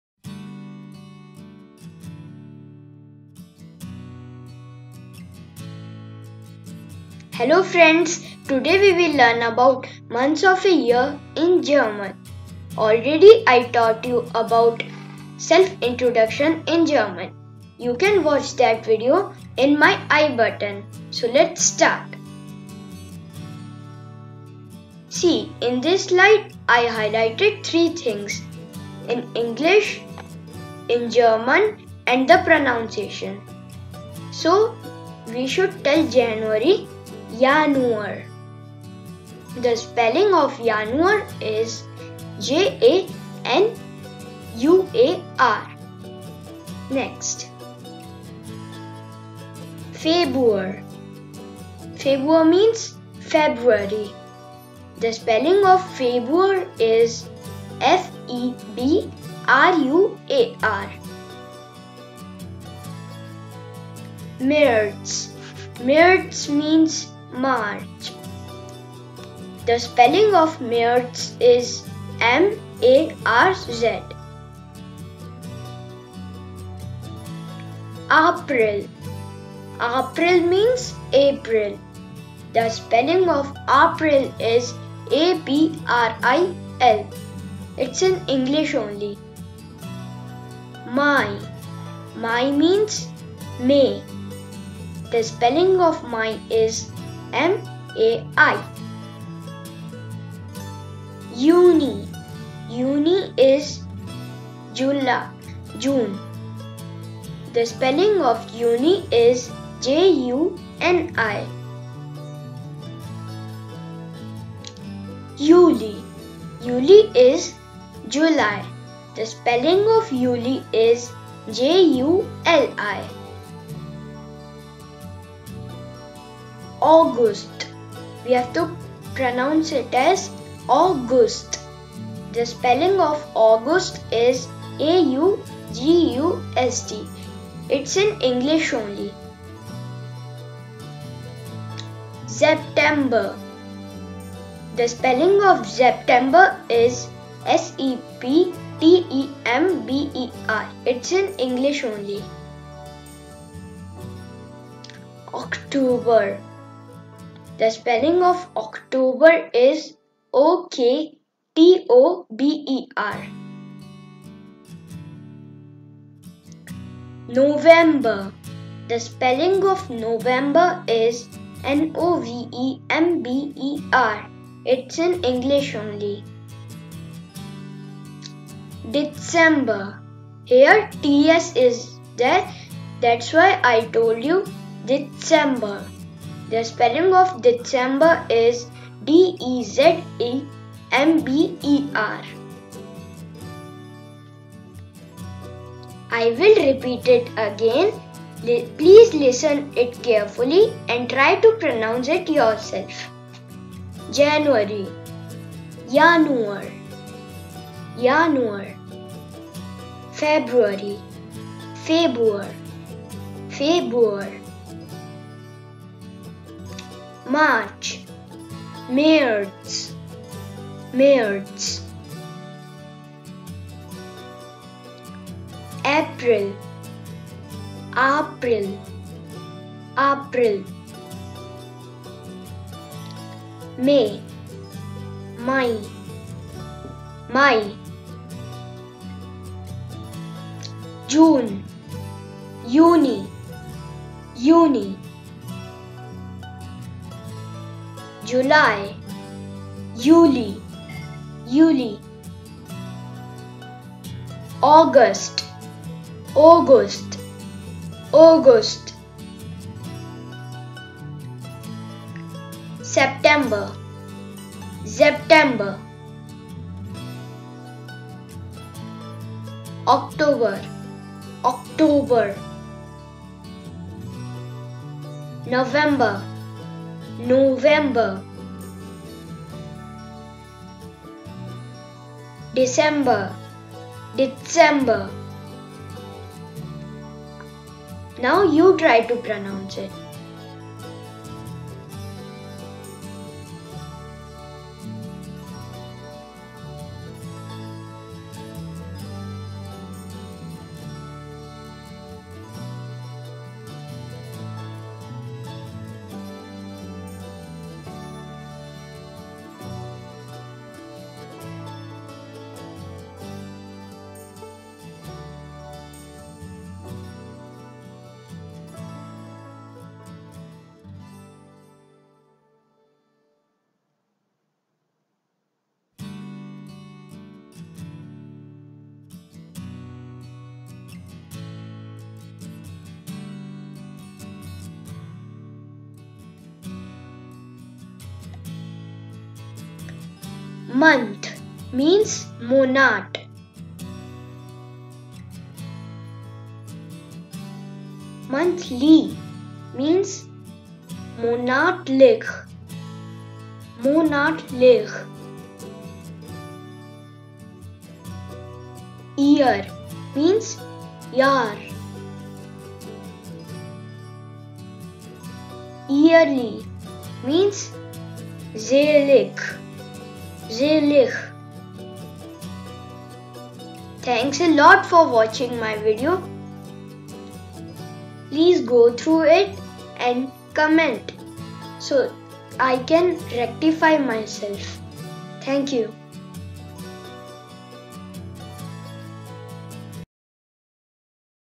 Learn German |Lesson 2| How to say the months of a year in German |German for Beginners | Vocabulary
This video is for beginners who learns German. In this I tried to say how to say months of a year in German.

You can also view : Learn German | How to Introduce Yourself in German | German for Beginners | Lesson 1 | Easy Learning